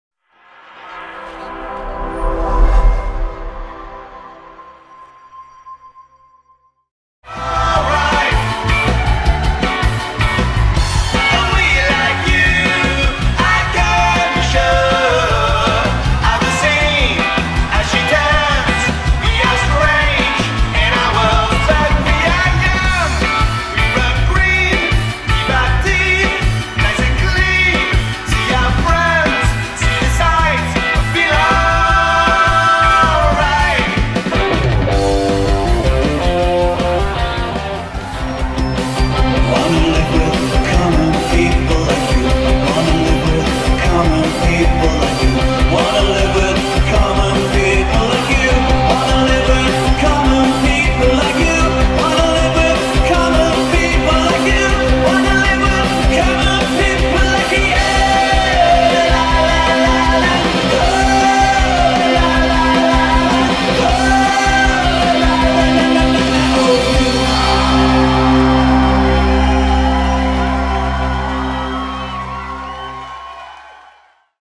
Britpop Forever Showreel - Available from AliveNetwork.com
Let Britpop Forever take you back to some of the greatest moments in British musical history! With their swagger & style, this band will have you singing and dancing your heart out to 90's Britpop hits from Blur, Pulp, Oasis & many more!